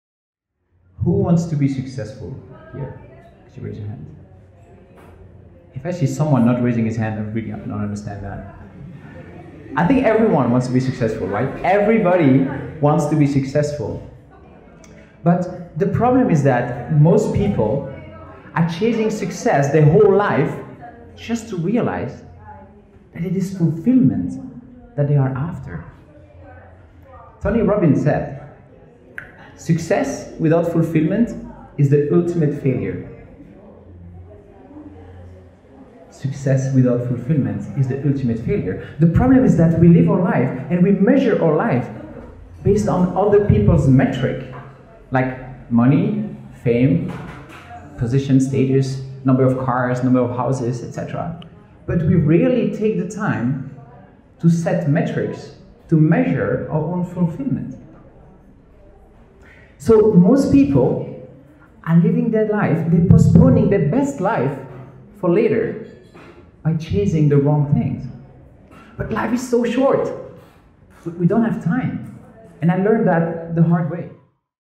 Stop chasing success when it is fulfillment that you are after!
Intro talk:
We measure our lives with the wrong metrics...

François de Neuville
Certified High Performance Coach